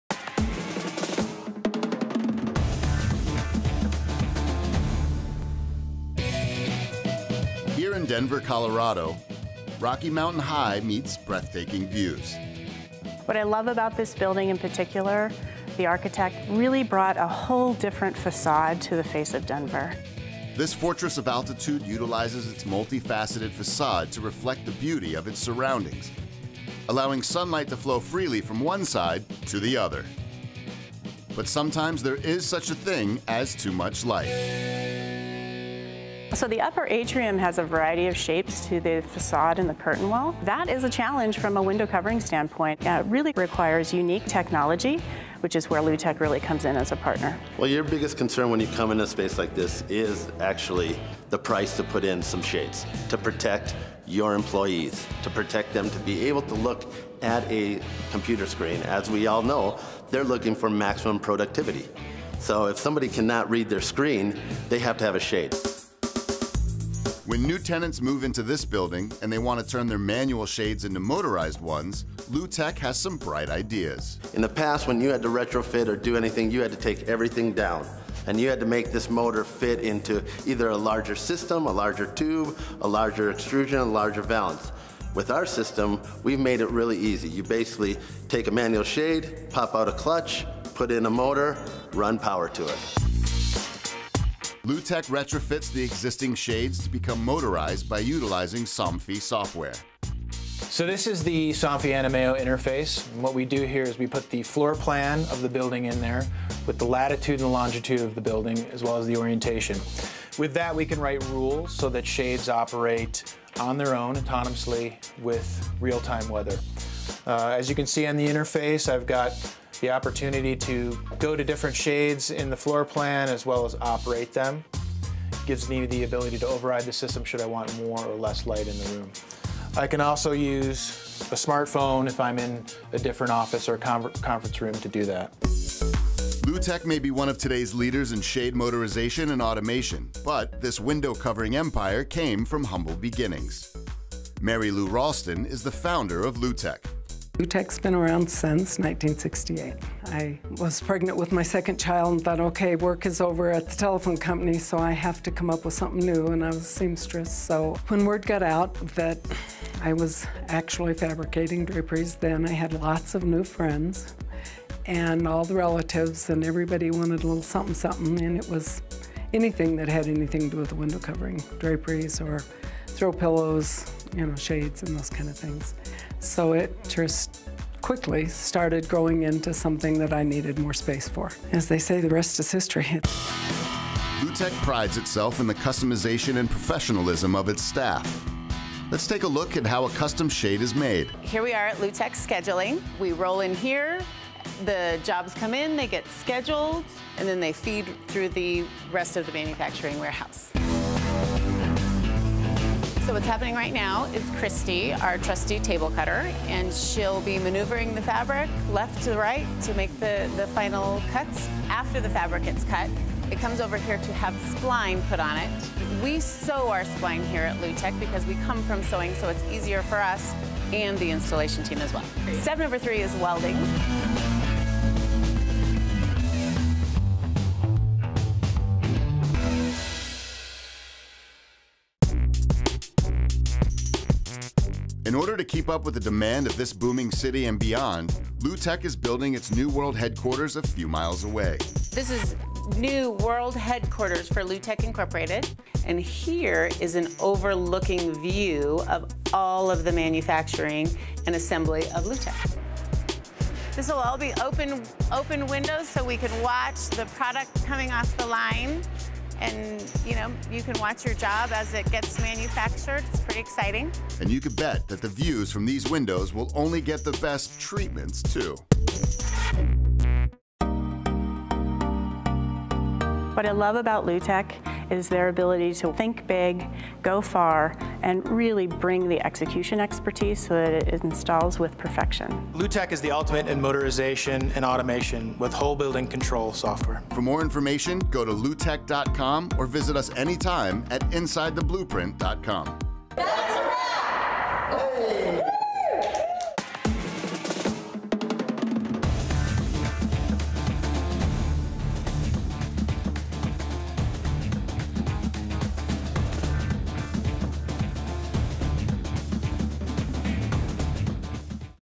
Let In the Perfect Amount of Sunlight with Lu-Tek
There’s nothing like the feeling of warm sunlight peeking through your living room windows. But sometimes, there is such thing as too much light. On this episode of Inside the Blueprint, we travel to Denver to meet with Lu-Tek.

From housing to towering skyscrapers, Lu-Tek is a frontrunner in window coverings and textiles to create fashionable and functional window shades. The process is simple: Lu-Tek retrofits the existing shades to become motorized with an exclusive software.

Founded in 1968, Lu-Tek has become one of the nation’s leaders in shade motorization. To keep up with demand, the company is in the midst of building a new world headquarters in Colorado. You’ll see it here first on Inside the Blueprint.